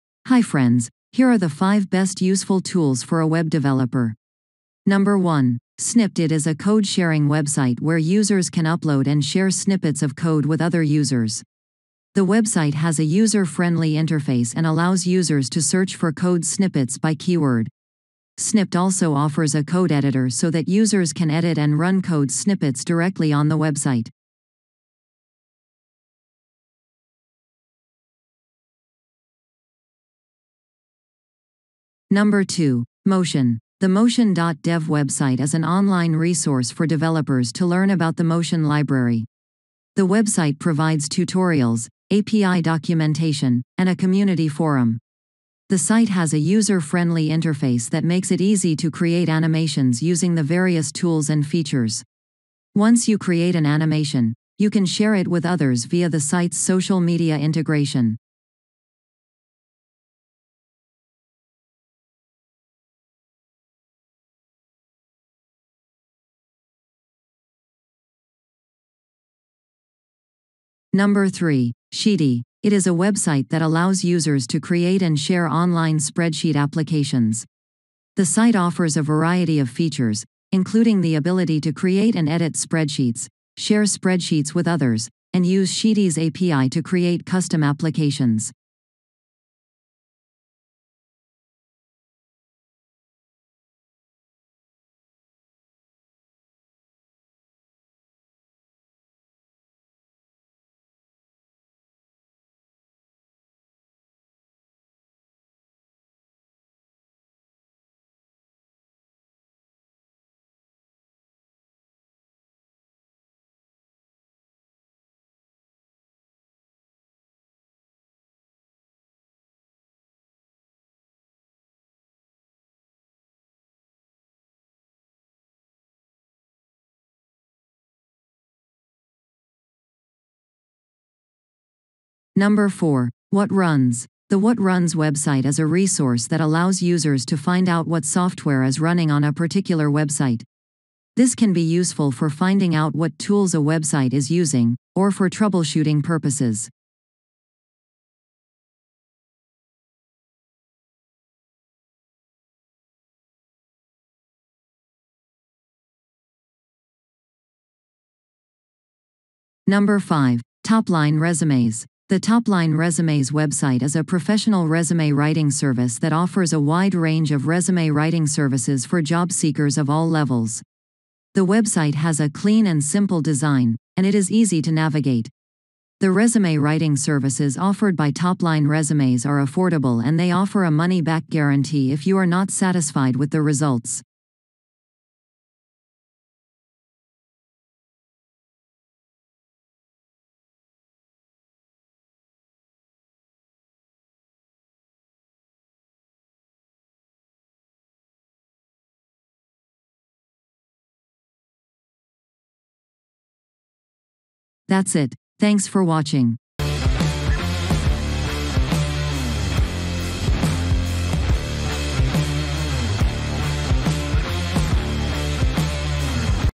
5 Best Useful Tools for a Web Developer 2022
Hi friends, Here are the 5 Best Useful Web Developer Tools 2022.

1. Snipt. It is a code-sharing website where users can upload and share snippets of code with other users. The website has a user-friendly interface and allows users to search for code snippets by keyword. Snipt also offers a code editor so that users can edit and run code snippets directly on the website.

2. Motion: The motion.dev website is an online resource for developers to learn about the Motion Library. The website provides tutorials, API documentation, and a community forum.

The Motion website is a simple, yet effective way to create and share animations online. The site has a user-friendly interface that makes it easy to create animations using the various tools and features. Once you create an animation, you can share it with others via the site's social media integration.

3. Sheety: Sheety is a website that allows users to create and share online spreadsheet applications. The site offers a variety of features, including the ability to create and edit spreadsheets, share spreadsheets with others, and use Sheety's API to create custom applications.

4. WhatRuns: The WhatRuns website is a resource that allows users to find out what software is running on a particular website. This can be useful for finding out what tools a website is using, or for troubleshooting purposes.

5. Topline Resumes: The Topline Resumes website is a professional resume writing service that offers a wide range of resume writing services for job seekers of all levels. The website has a clean and simple design, and it is easy to navigate.

The resume writing services offered by Topline Resumes are affordable and they offer a money-back guarantee if you are not satisfied with the results.

Chapters:
00:00 - Intro
0:05 - 1. Snipt
00:32 - 2. Motion
01:11 - 3. Sheety
02:12 - 4. WhatsRun
02:40 - 5. Topline Resumes